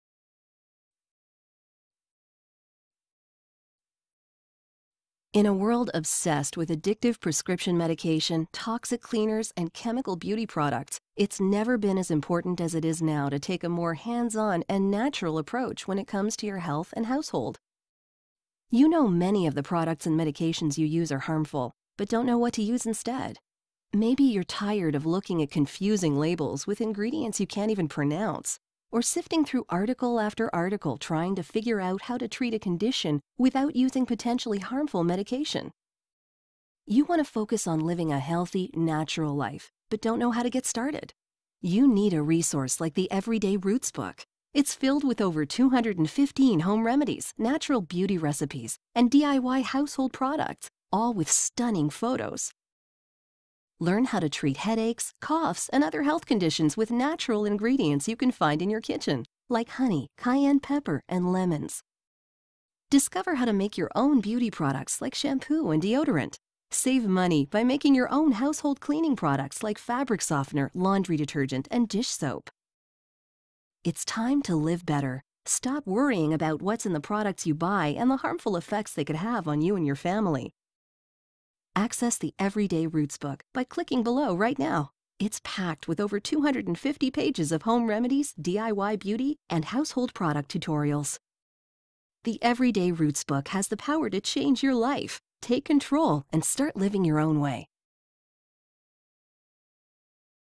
How to replace the toxic products - Everyday Roots Book
Get instant access to the guide go:
PROTECT YOUR FAMILY FROM TOXIC PRODUCTS AND MEDICATIONS
Learn how to replace the toxic products and medications in your home with over 215 healthier, all-natural alternatives.